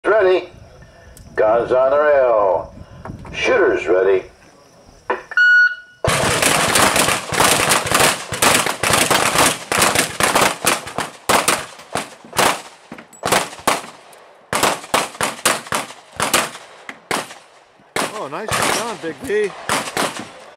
2024 The Pin Shoot: "The Kid" shoots the revolver optional event for the first time.
Eight pins, six shots. Even if you use an eight-shot revolver, there is a mandatory reload so it might as well be a Model 25 in .45 ACP for fast reloading. He placed third actual but got second pick at the prize table due to not being a handgun Master Blaster. At TPS there are Master Blasters and "OSS", Ordinary Standard Shooters. At the awards the winner of an event gets first pick, but second pick goes to the first OSS.

This was not his winning table but darned good reload for never having done it before!

He was made a long gun Master Blaster last year, and a handgun Master Blaster this year.